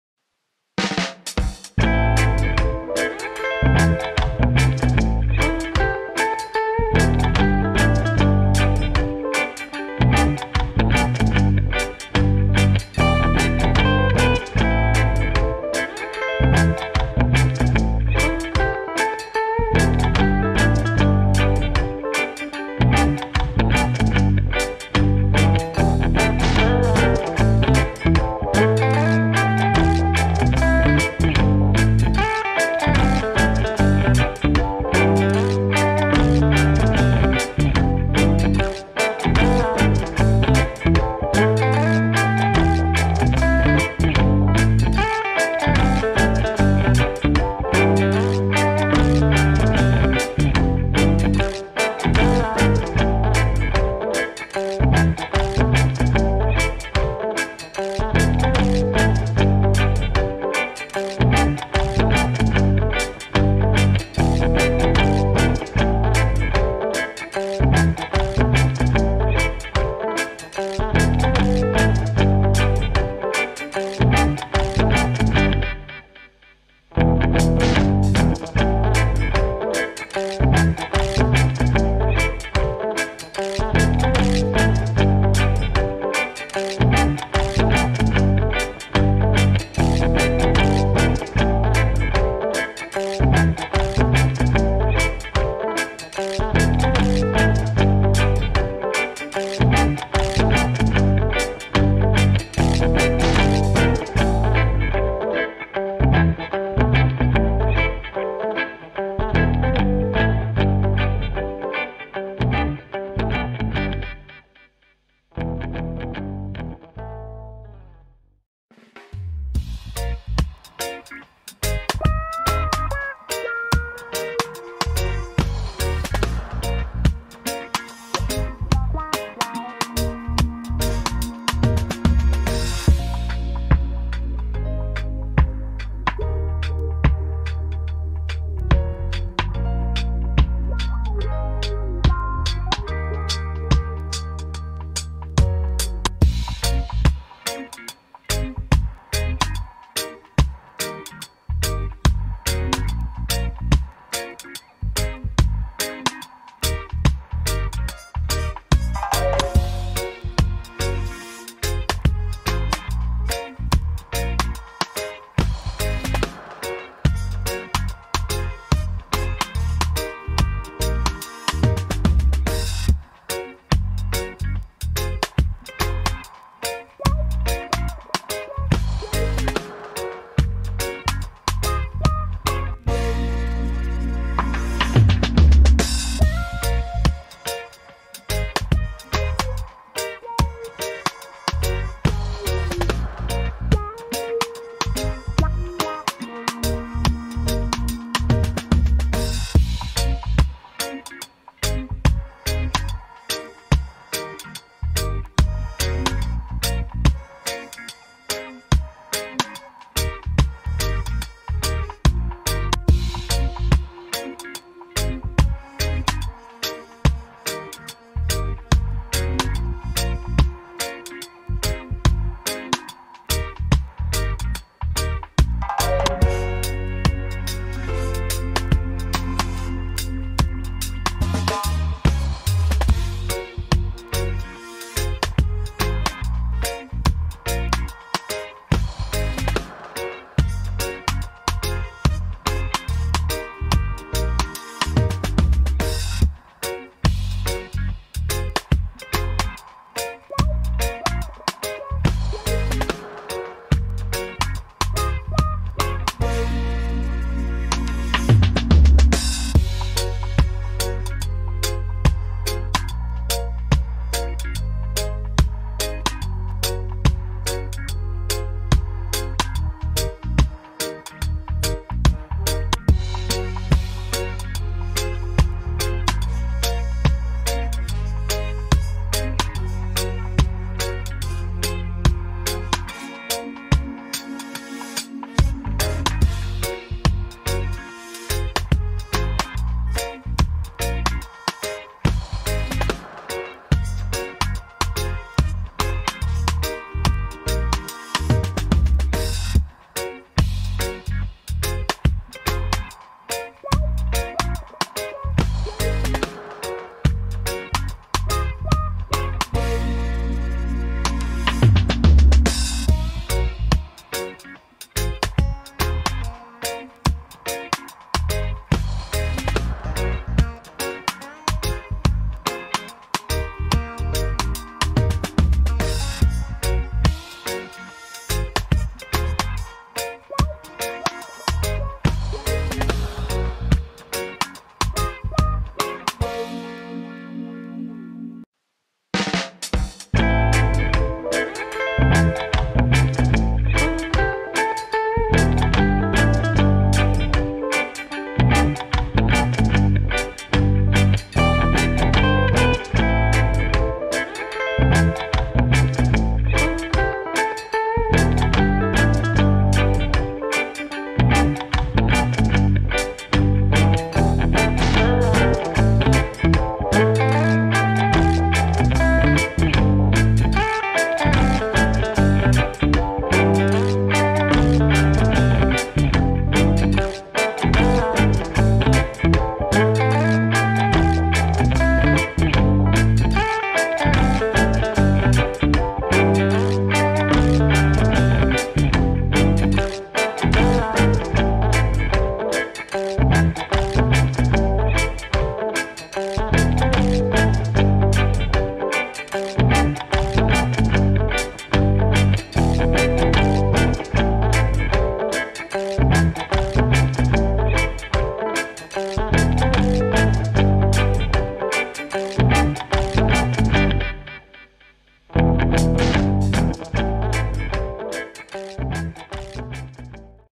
Bob Marley 1/6 Scale Figure Review Video
Video Review of Win C Studio Legendary Pacifist Singer (Bob Marley) Figure.
Photos and Video of the contents of the package and figure.